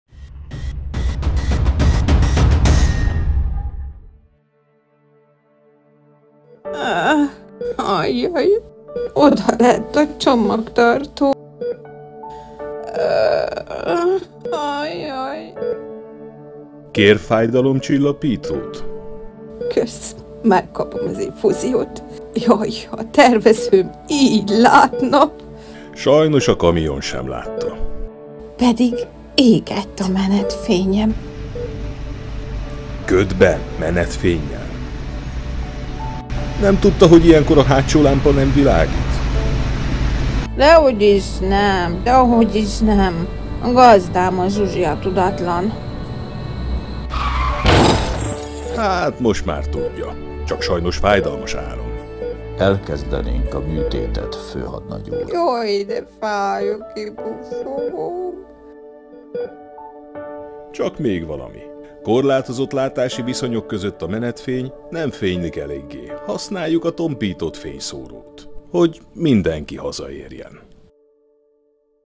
ZsandÁrpi aktái 8. epizód
A baleset megelőzési sorozat újabb epizódja azt mutatja be ZsandÁrpi rendőr-főhadnagy segítségével, hogy miért balesetveszélyes korlátozott látási viszonyok mellett kizárólag a menetfény használata.